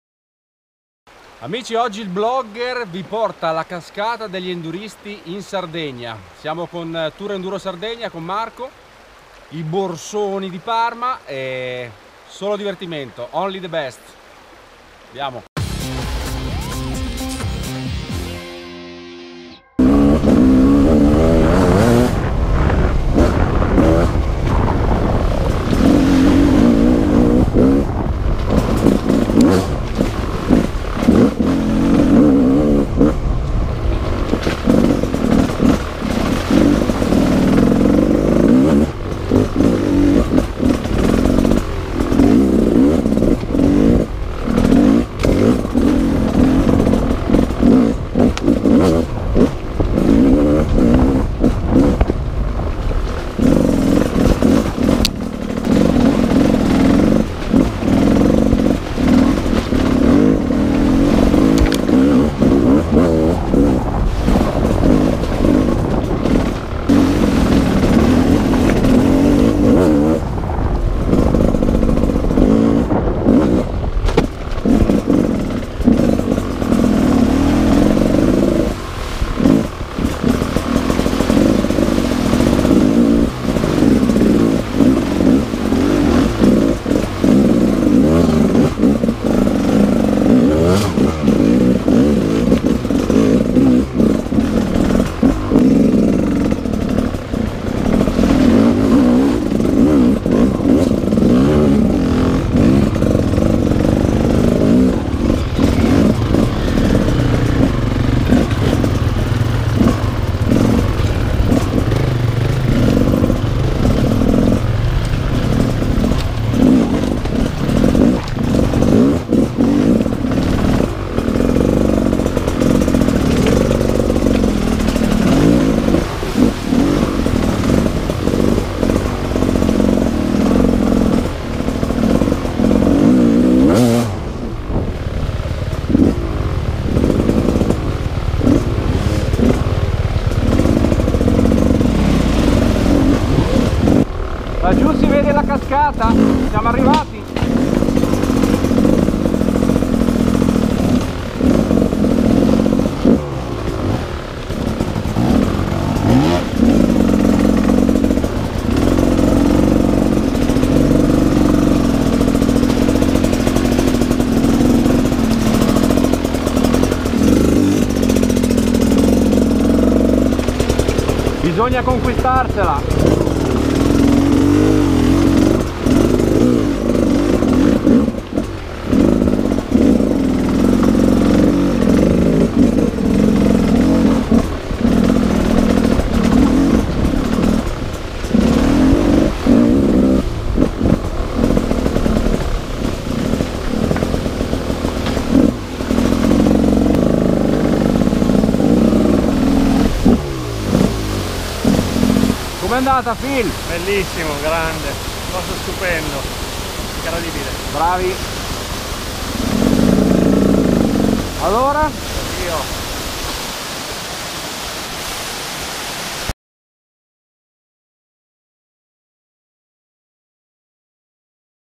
NEW GAS GAS EC 300 2019 Six Days | First ride
Con la nuova GasGas EC 300 2019 Six Days
In Sardegna con Tour Enduro Sardegna alla scoperta della cascata degli Enduristi!!

"Se lo sai lo sai, ma se non lo sai te lo dice il Blogger"